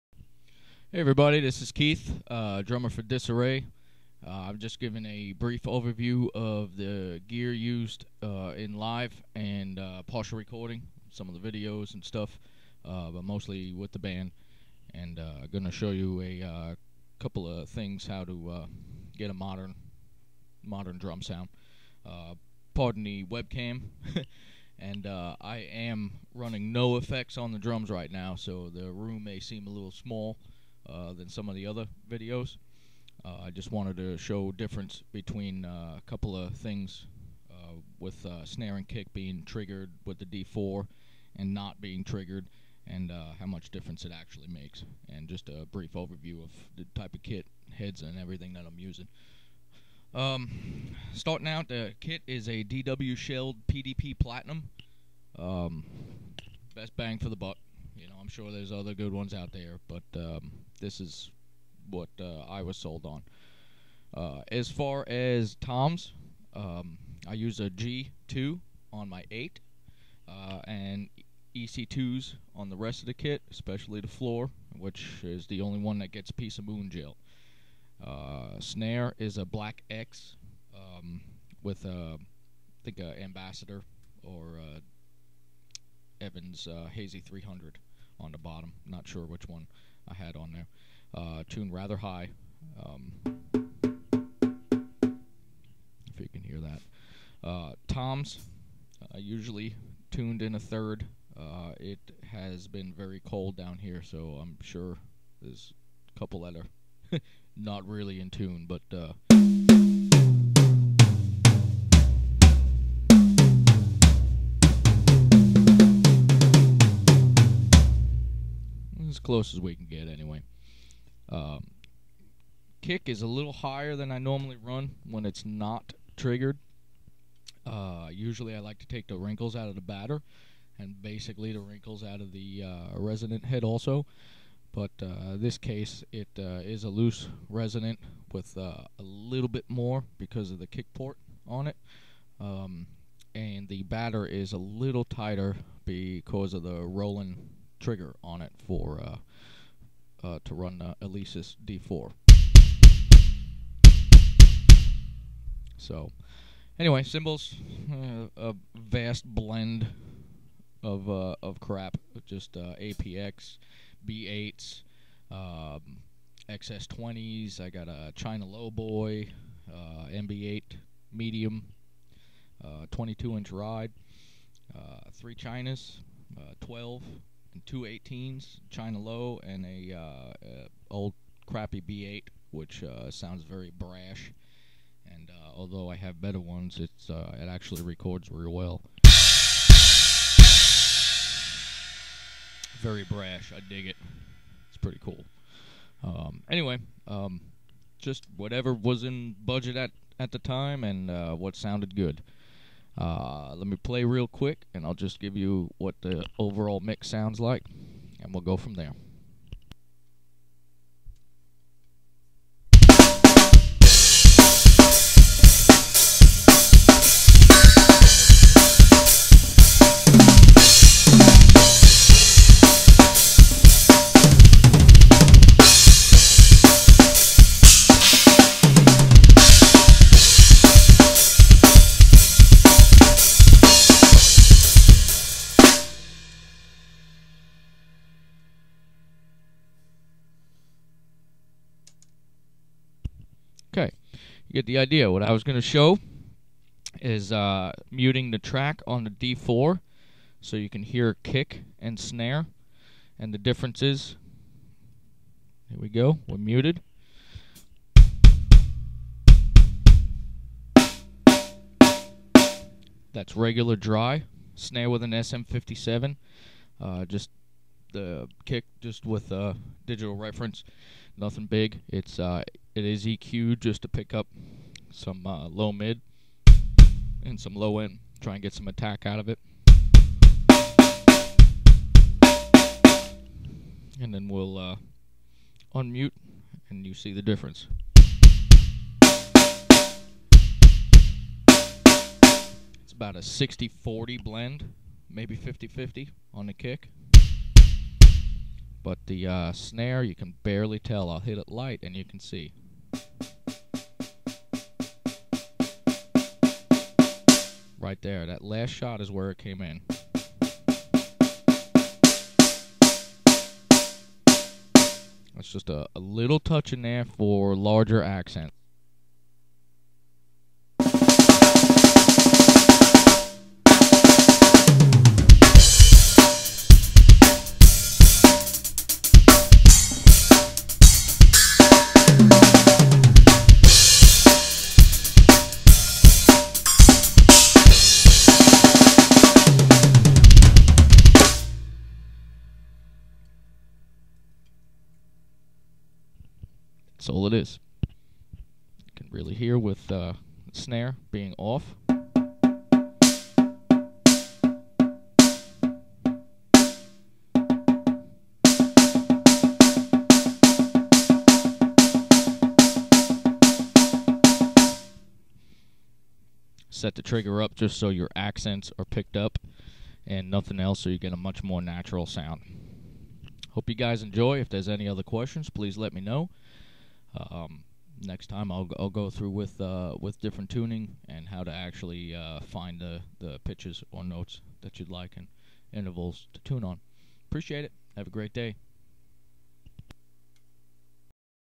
Keith Feulner Disarray Drummer-Trigger & Setup Review
Here is a basic overview of my kit and alessis d4 drum trigger system. i use a simmons piezo on snare and roland kick trigger. im no expert instructor so easy on the comments. just sharing to people who care to learn. not the cleanest or the fastest, but im just getting the idea accross. shame we have to write stuff like this, but you know have the youtube bashers are. keith